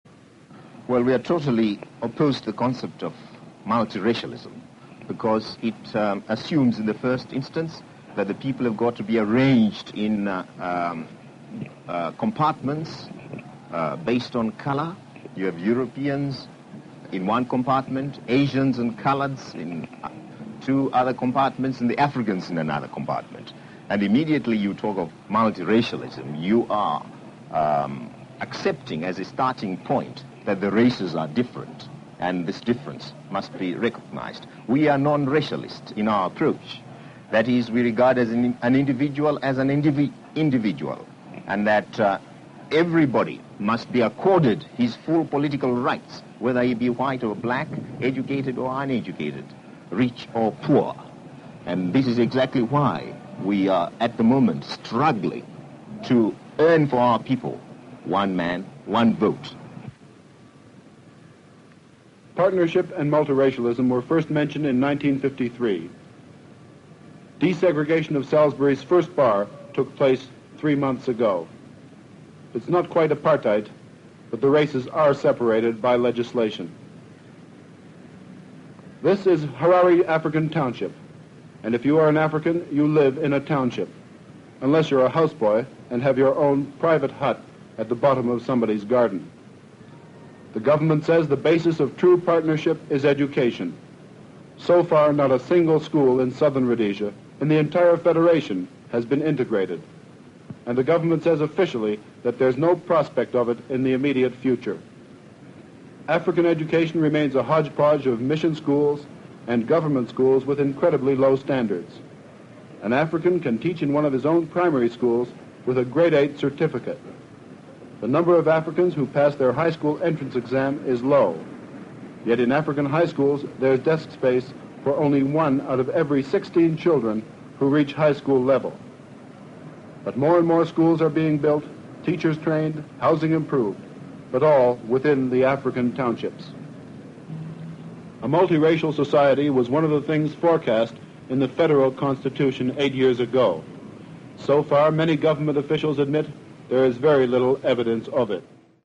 Young President Robert Mugabe
One of earliest videos of His Excellency President Robert Mugabe.